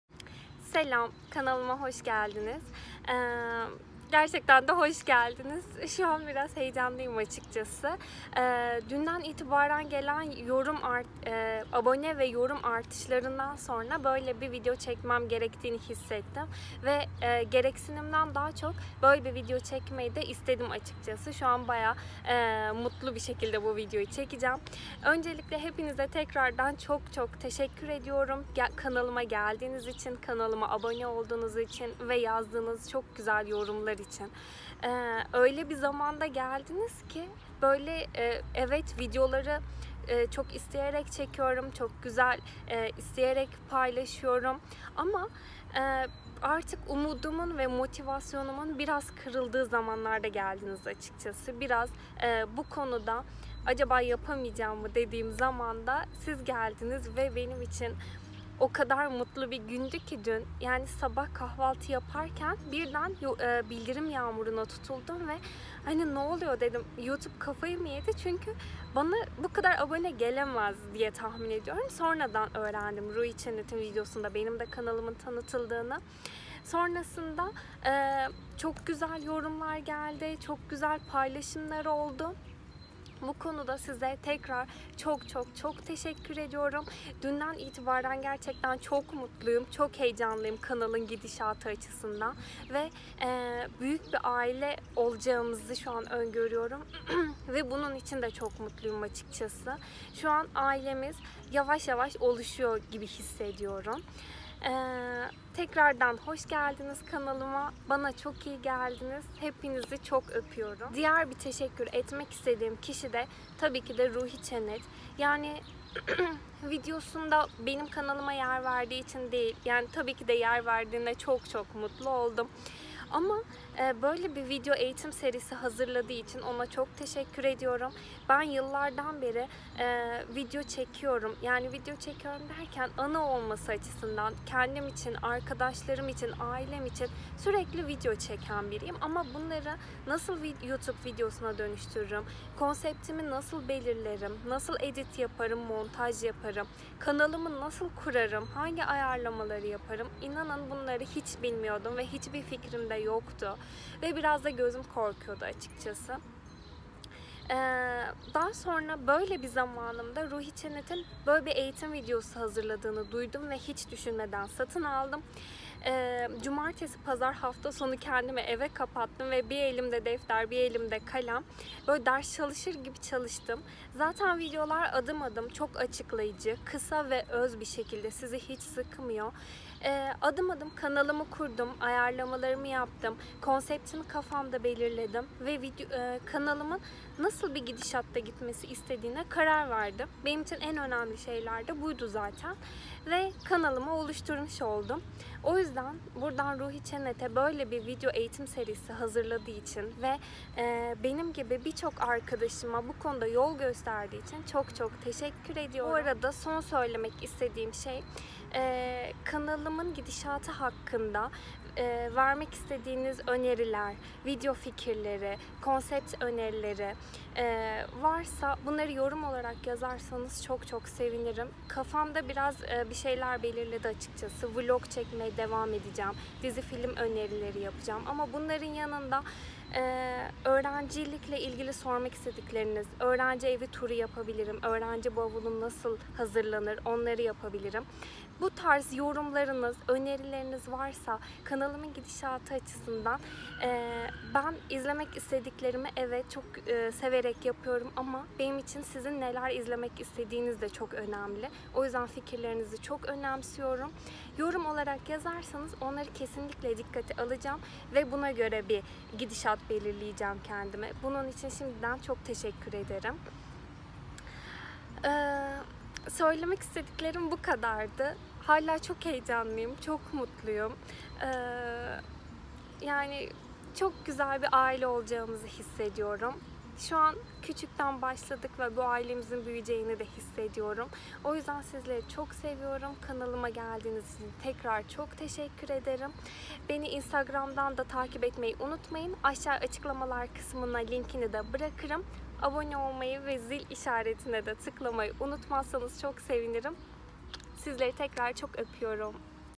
HEPİNİZ HOŞ GELDİNİZ!!
Ruhi Çenet'in kanal tanıtımı videosunda benim de kanalıma yer verdikten sonra hiç beklemediğim kadar bir abone artışı oldu. Ben de öncelikle sizlere bir hoş geldiniz demek istedim:)
Video önerilerinizi bekliyorum!